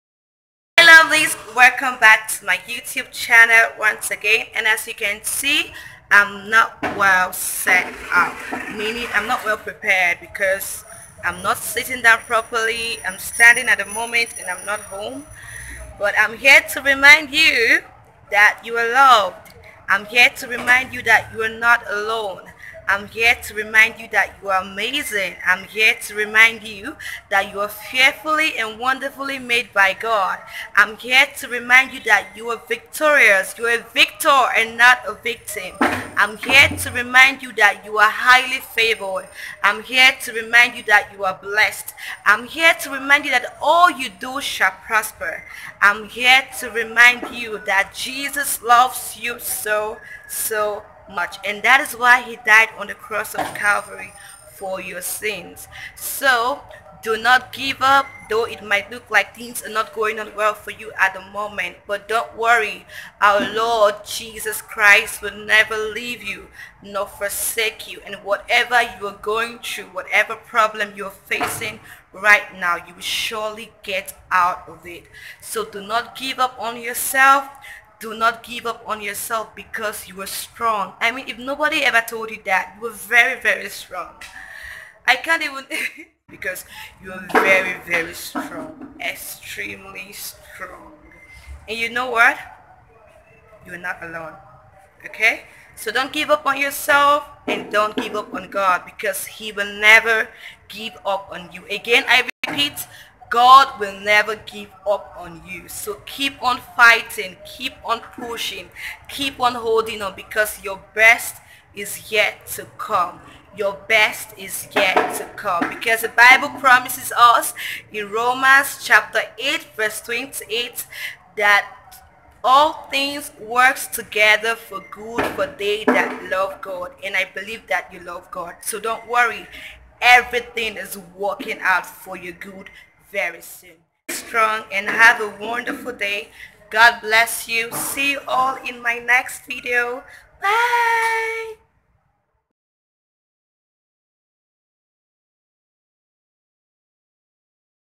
YOU ARE LOVED| ROMANS 8:28| REMINDER
Hello Lovelies

Truth be told, in a world were everyone is ON THEIR OWN.

We all need people or at least someone to remind us that we are loved, that we are fearfully and wonderfully made by God, that we are amazing and that we are strong.

This video might not cover all the affirmations you need or want to hear but I pray the little that were stated puts you back on track.

All that was said, was from my heart and I pray it blesses you.